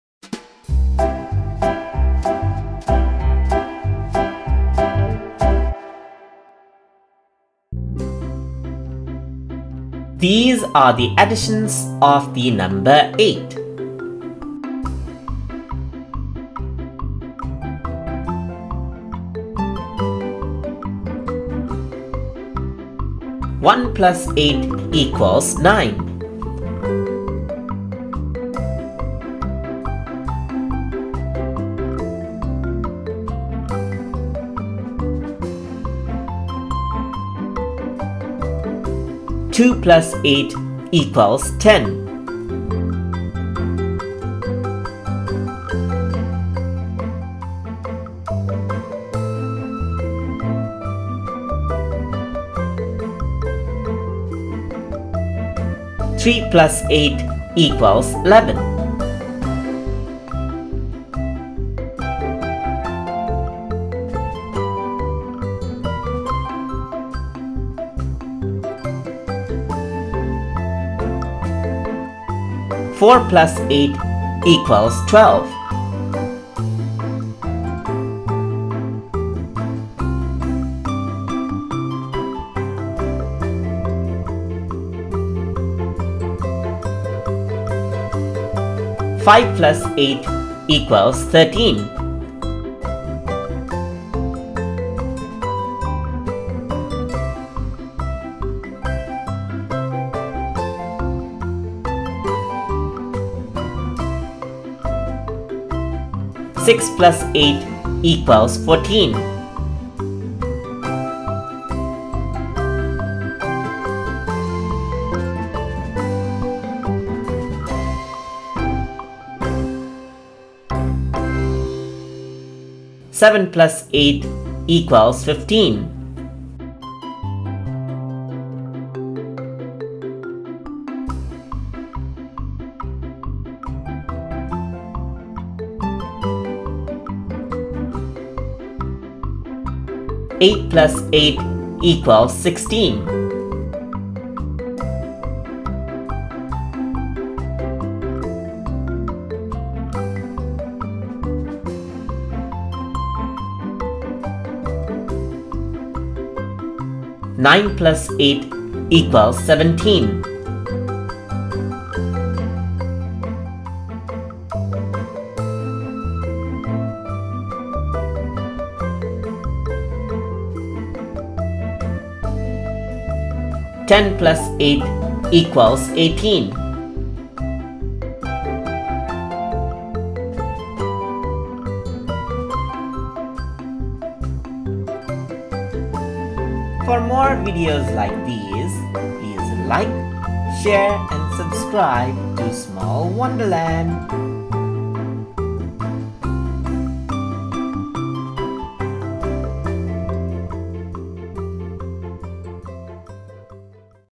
Additions of 8
In this video, we are going to learn about the additions of the number 8.